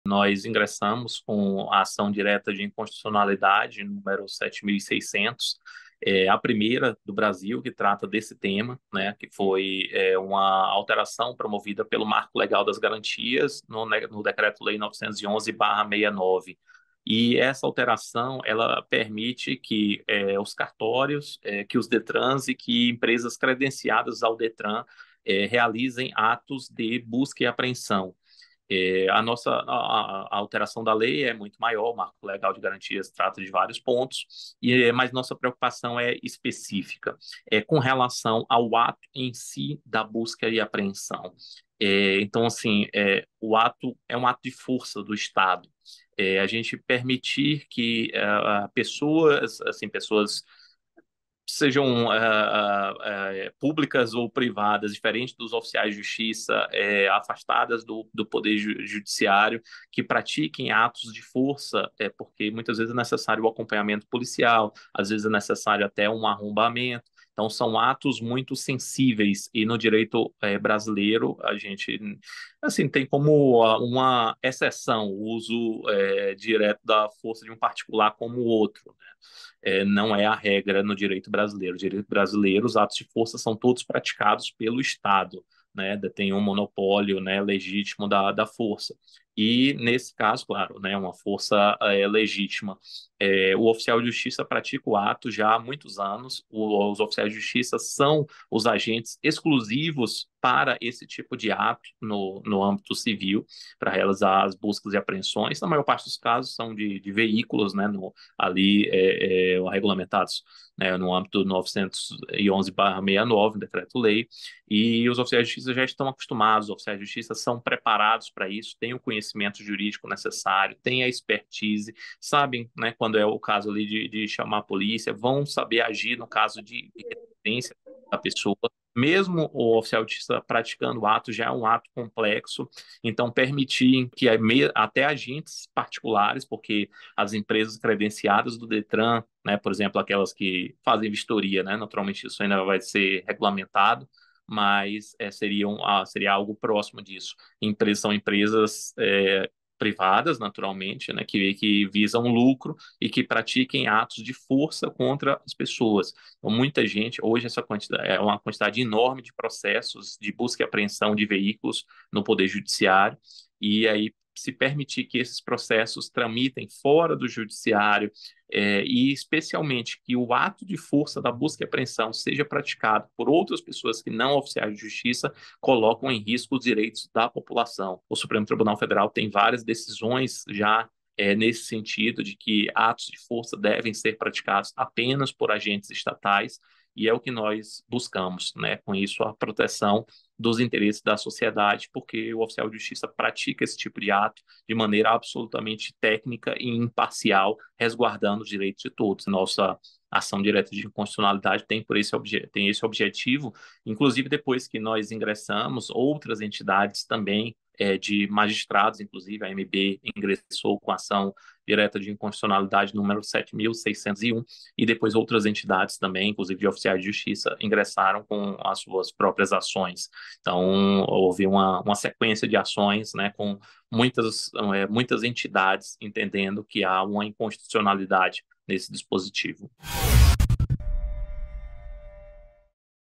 Presidente da UniOficiais explica ação no STF contra busca e apreensão de veículo sem ordem judicial
Presidente da UniOficiais, Gerardo Alves Lima Filho explica ADIn no STF contra a possibilidade de busca e apreensão de veículos sem ordem judicial, prevista no marco legal das garantias.